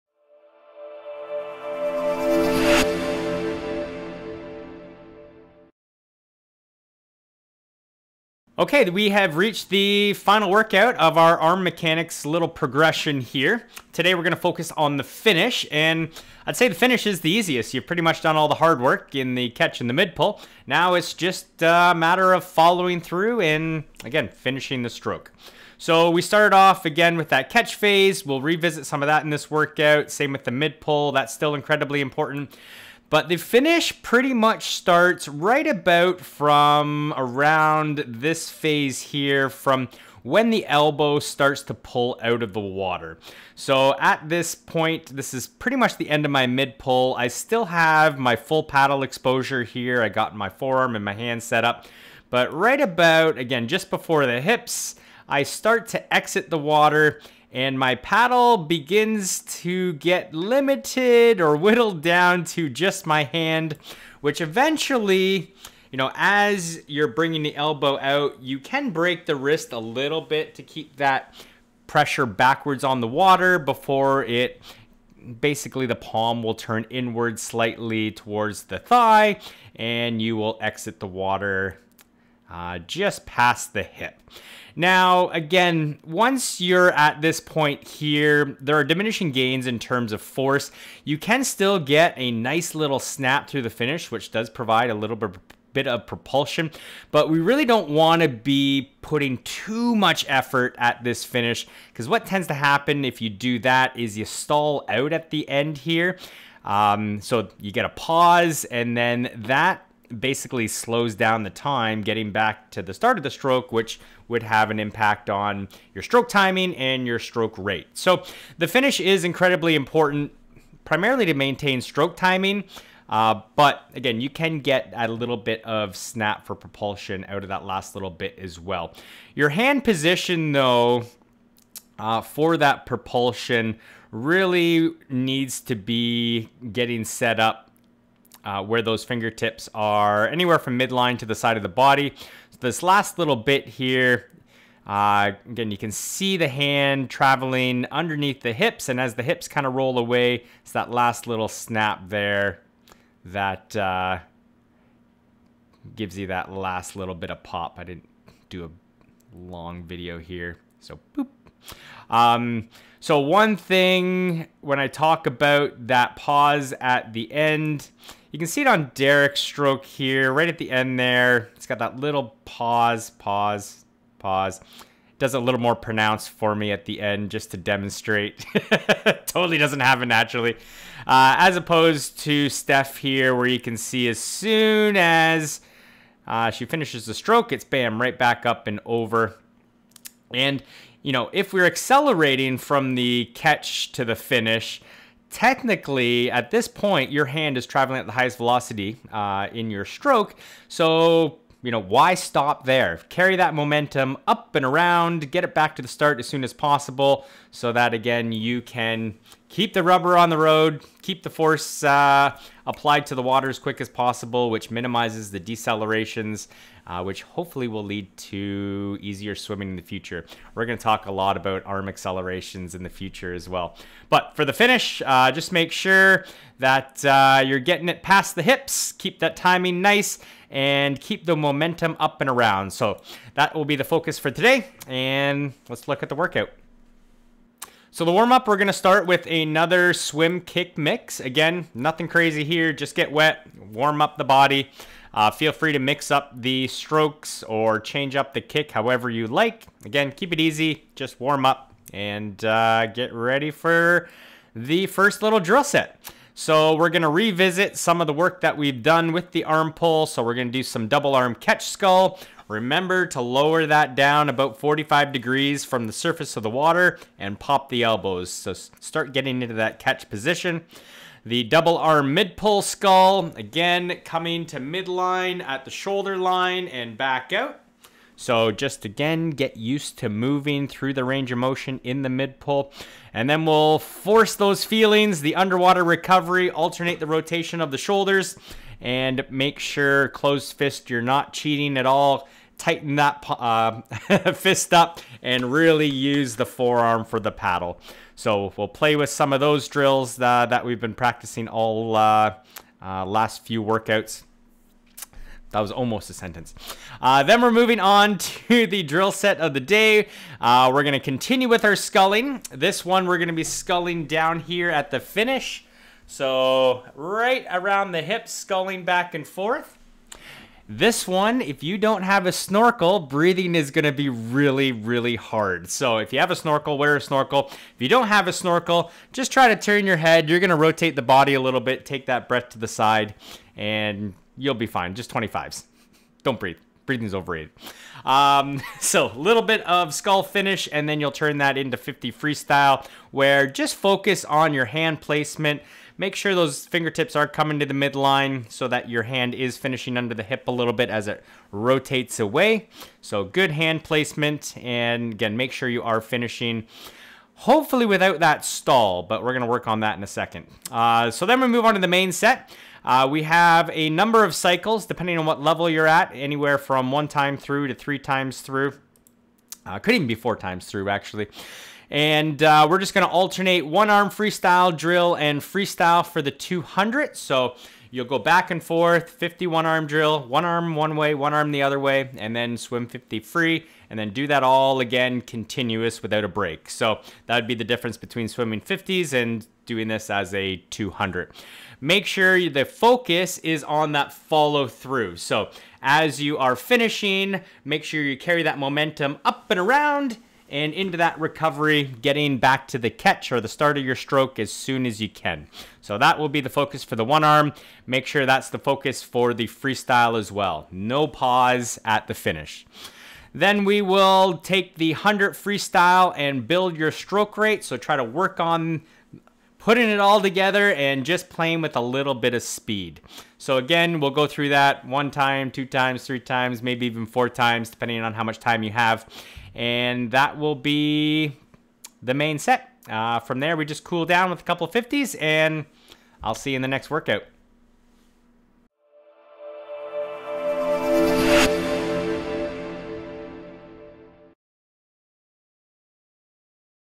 Finish Fast! Don't Get Stuck At The Finish...
Finish fast and follow through! This workout will focus on the finish of your freestyle arm pull.

M O R E    V I D E O S    T O    W A T C H    N E X T :